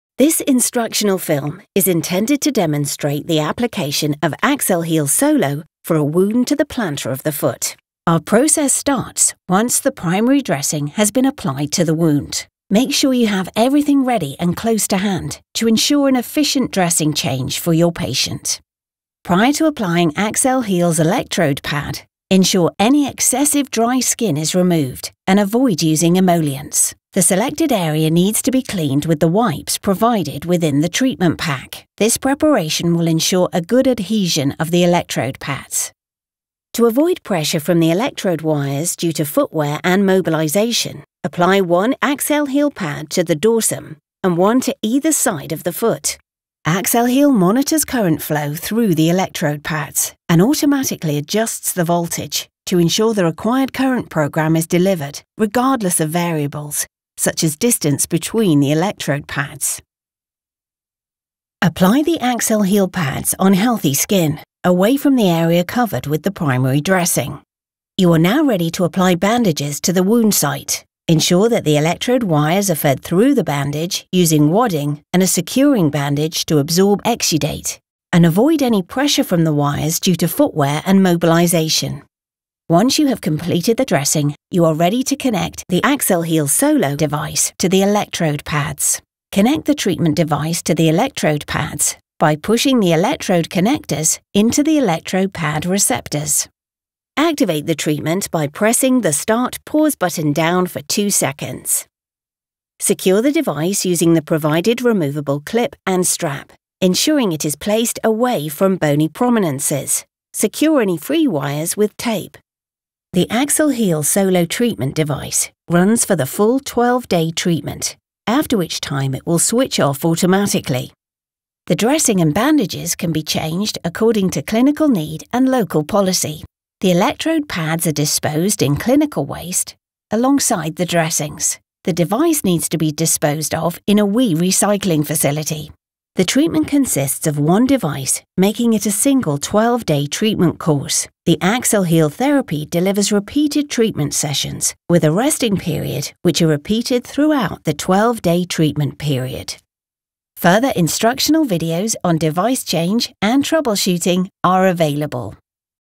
How to use Accel-Heal Solo on a sole of foot wound
Liz Ovens, Independent Tissue Viability Specialist Nurse, describes how to use the Accel-Heal Solo product on different bandage types.